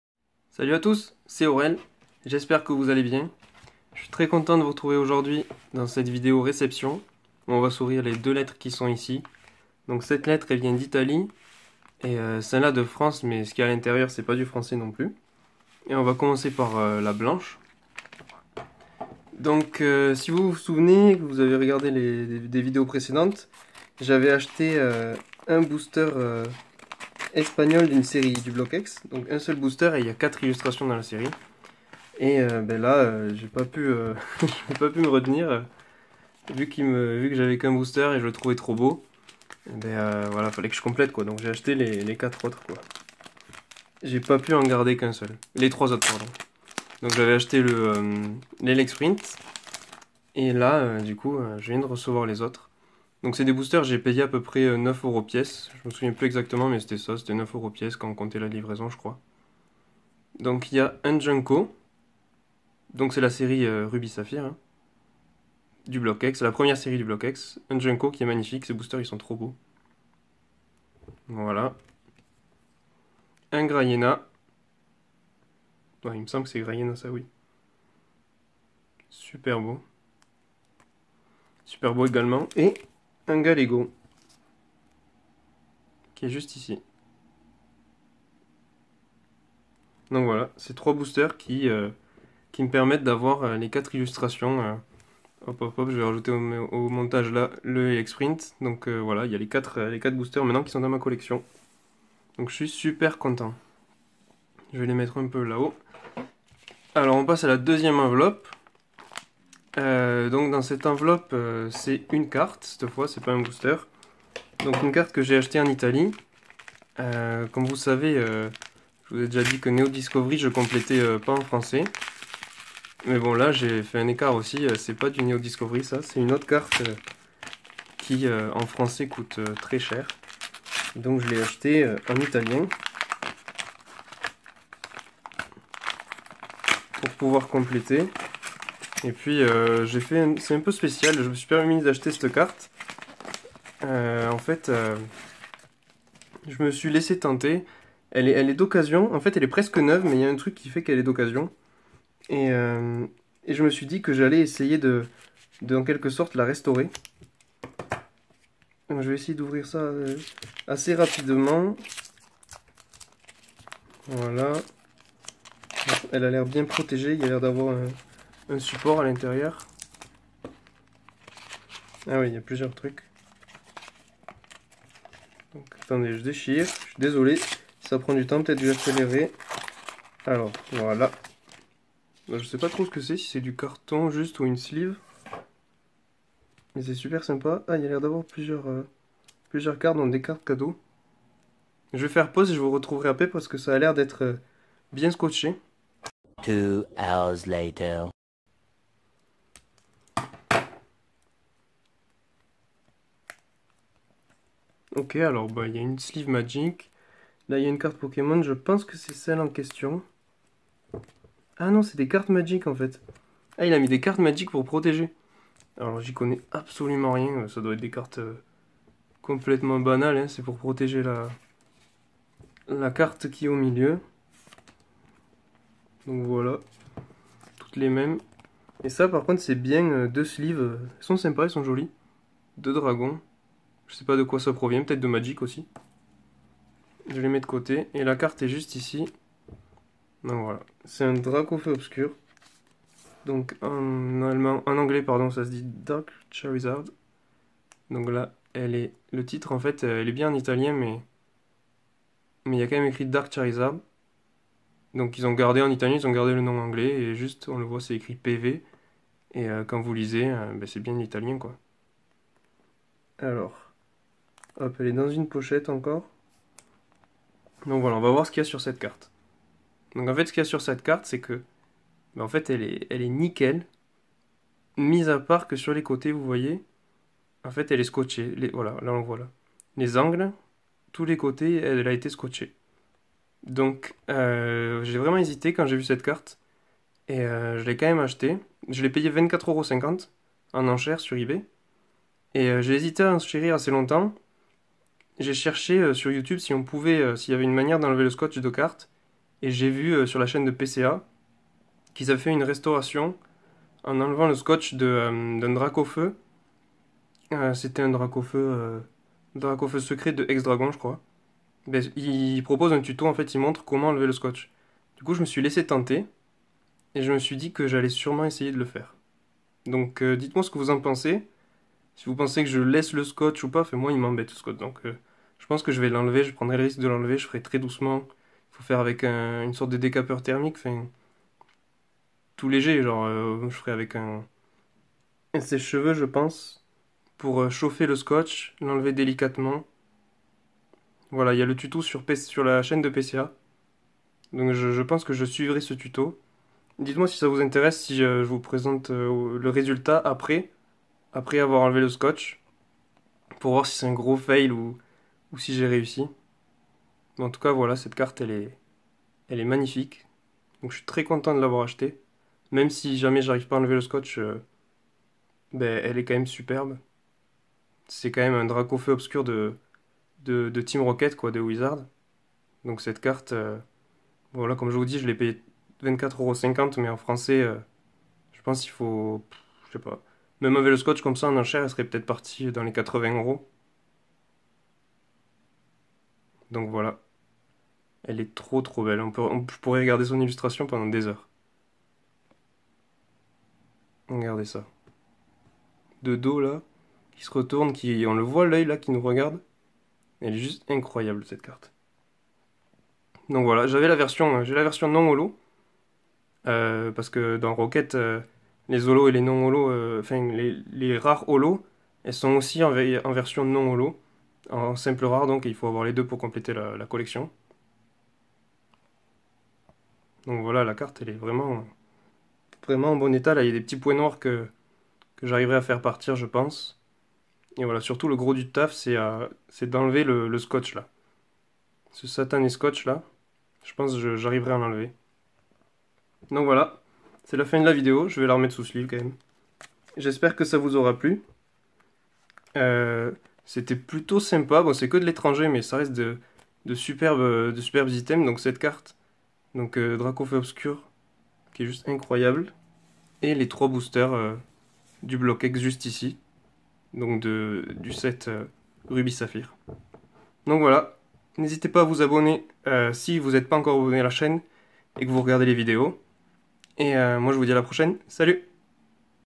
Réception d'une SUBLIME CARTE POKEMON WIZARDS DRACAUFEU et de BOOSTERS EX !
Salut !

Je partage aujourd'hui dans cette vidéo la réception d'une carte Pokémon que je cherchais à me procurer depuis un petit moment, et de 3 boosters du bloc Ex qui me permettent d'avoir le set en scellé au complet :)
Comme vous verrez je me suis lancé un petit défi concernant la carte en question !
Dites moi ce que vous en pensez en commentaire :)

Je te remercie d'avoir regardé la vidéo et lu la description :)